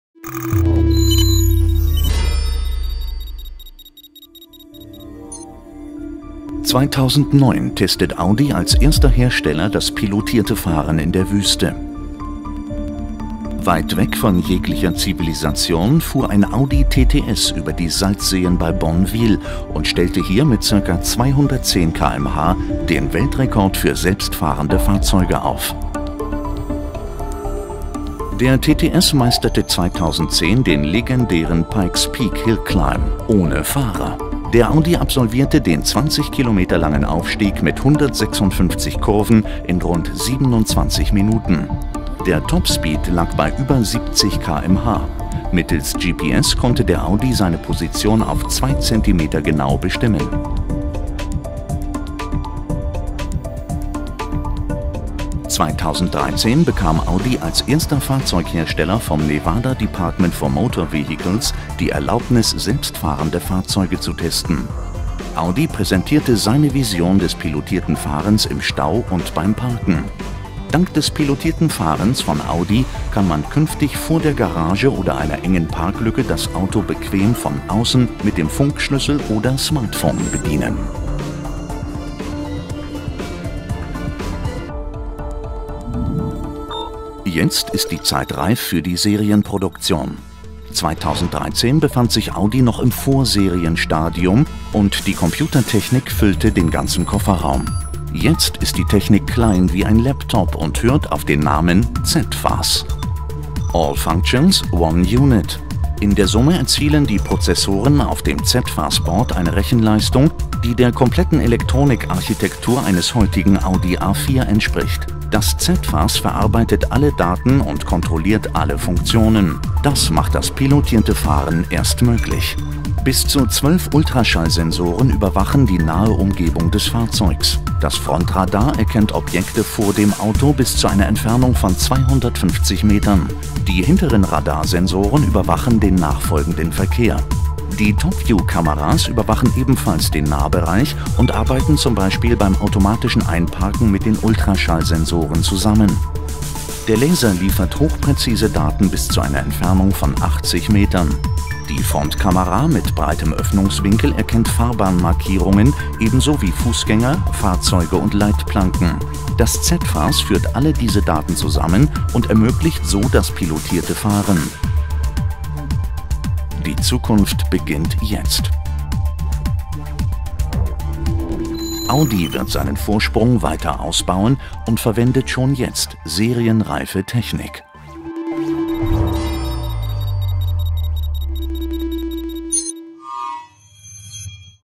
Pilotiertes Fahren bei Audi
2013 bekam Audi als erster Fahrzeughersteller in den USA die Erlaubnis selbstfahrende Autos zu testen. Schon jetzt verwendet das Unternehmen für das pilotierte Fahren im Stau sowie das pilotierte Einparken eine serienreife Technik. Während die Computertechnik 2013 noch den gesamten Kofferraum füllte, ist sie Anfang 2014 bereits klein wie ein Laptop und steuert die Sensoren und Kameras am Auto, die das Fahren ohne Fahrer ermöglichen.Mehr Infos zum pilotierten Fahren finden Sie auf dem Audi Blog  und den Audi MediaServices